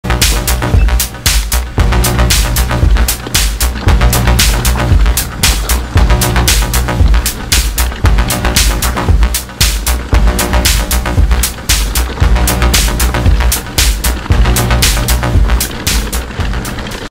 Trialera pedregosa Ebike
Comienzo trialera  por la zona de Sant Martí Sarroca